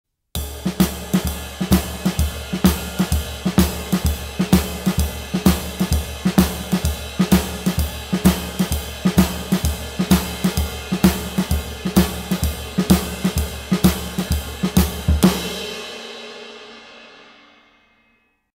Swing Shuffle Feel
Here's a pattern you can play if you're asked to play a shuffle.

This is an excerpt from the full-length lesson "Jazz Ride Cymbal Part III: 4 Feel and Shuffle."